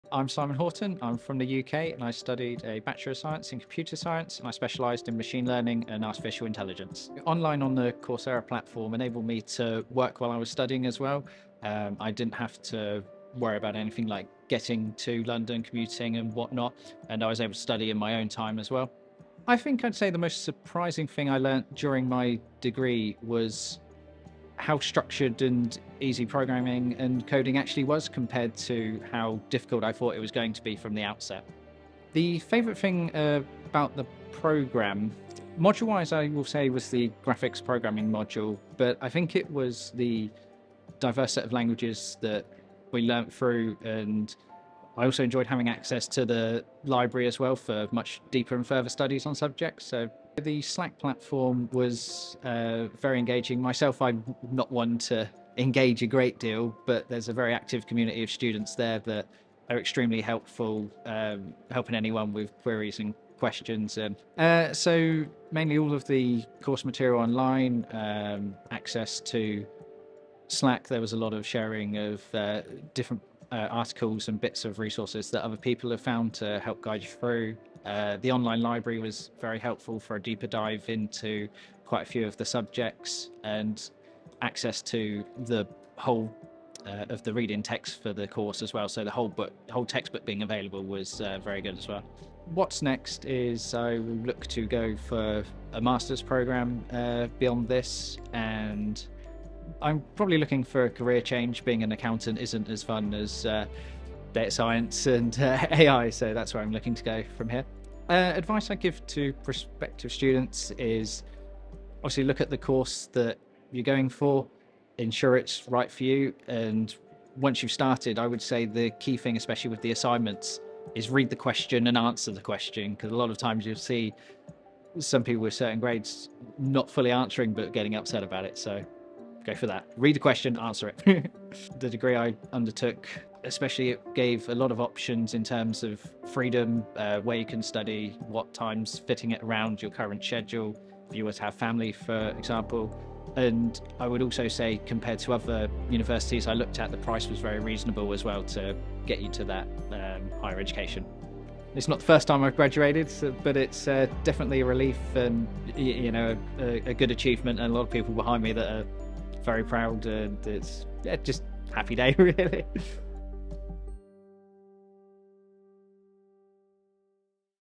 Alumni inspiration: Simon, BSc Computer Science (Machine Learning and Artificial Intelligence), UK
BSc Computer Science graduate, Simon, specialised his degree in Machine Learning and Artificial Intelligence. Simon was surprised at how easy and structured coding was, compared to how difficult he thought it would be. He enjoyed the Graphics programming module but more than this his most favourite aspect was the diverse set of programming languages that the group learnt.